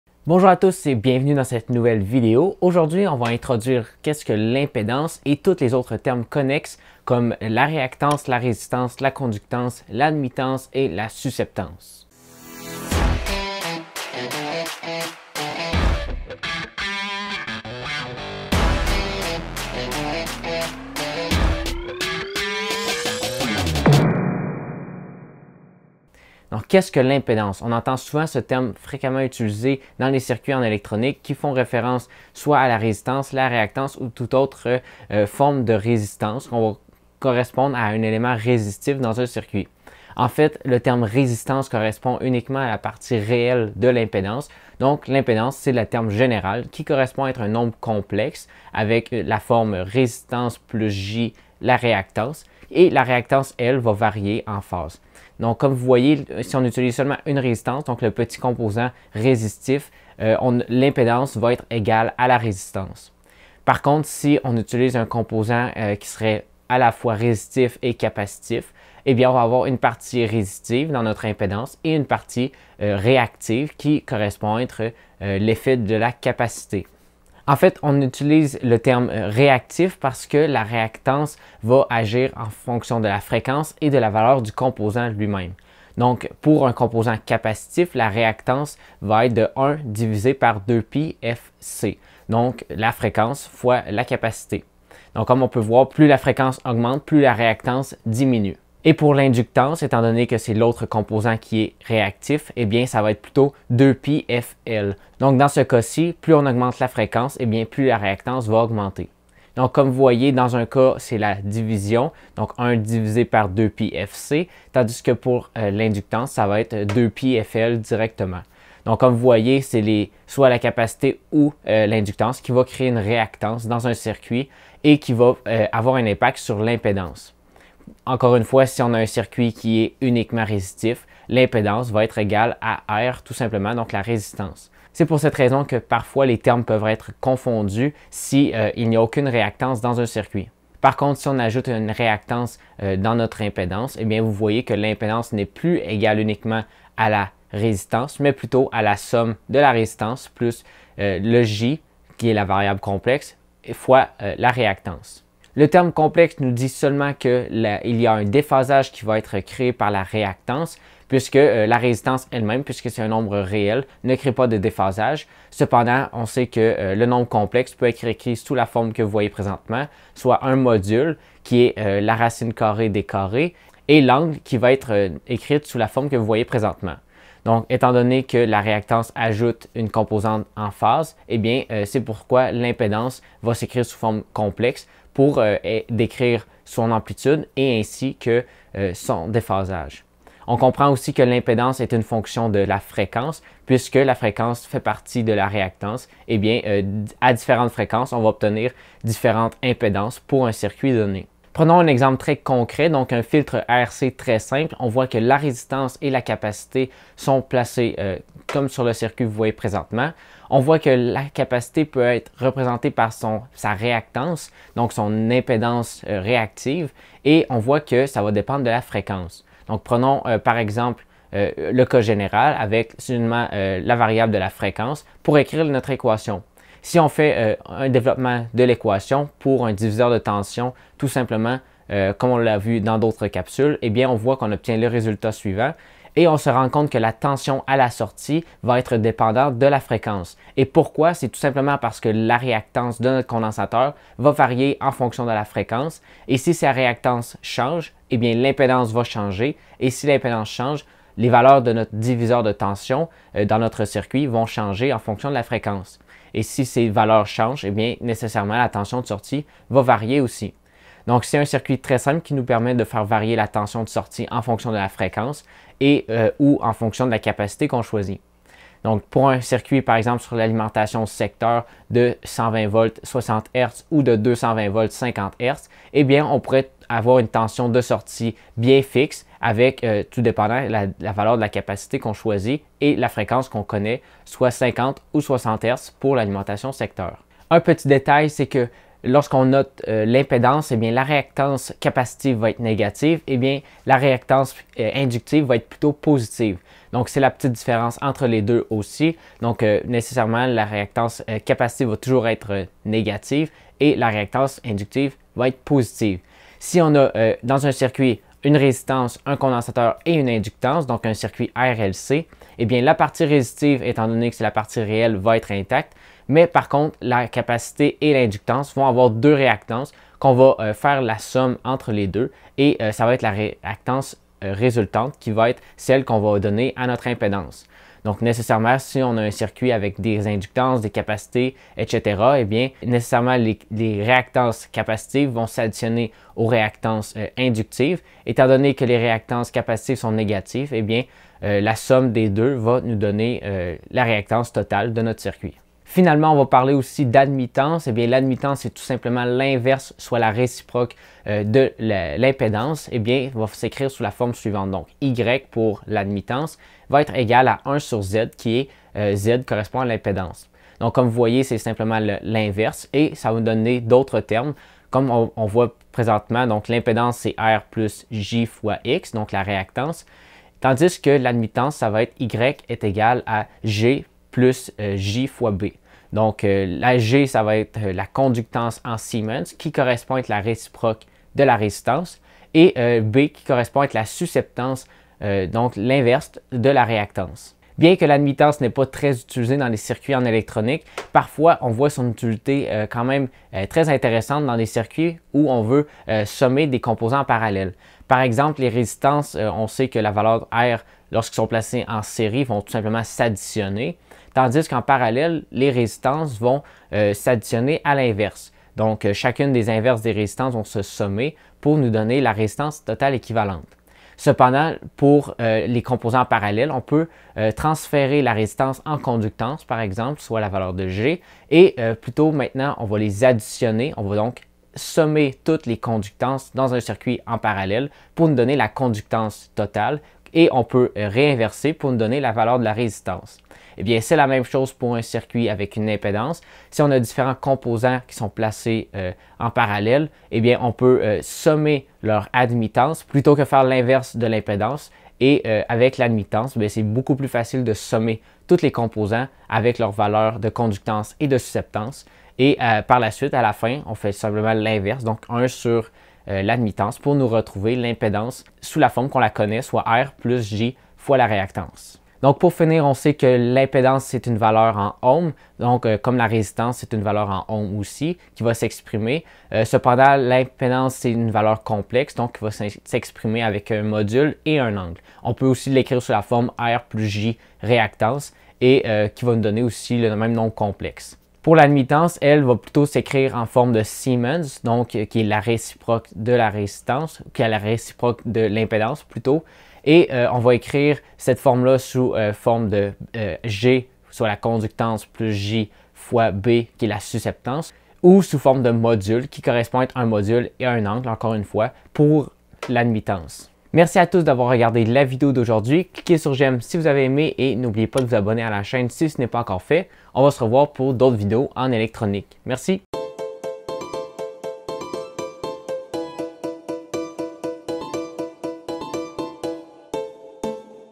Qu'est-ce que l'impédance? Admittance, susceptance, réactance, etc.?
L’impédance ou la résistance ? Les termes peuvent sembler vouloir dire la même chose, mais il faut faire attention puisqu’ils ont une signification différente. Dans la vidéo, je vous présente tous les termes et je vous présente ce qu’ils représentent dans les équations. La résistance est uniquement la partie réelle de l’impédance. Dans certains circuits particuliers, l’impédance sera égale à la résistance (uniquement pour les circuits où la réactance est nulle). On discute aussi de l’admittance, la conductance et la susceptance. Je vous aide à démystifier ces termes pour mieux comprendre leur utilisé.

Mon équipement vidéo :
Micro cravate
Caméra Sony A7ii
Lentille Tamron 28-75 mm f/2.8
Éclairage studio

0:00 Introduction
0:25 Qu'est-ce que l'impédance?
1:17 Réactance
3:21 Un exemple
5:06 Circuit RLC
5:46 Qu'est-ce que l'admittance?
6:28 Un exemple
7:42 Les unités
8:23 Conclusion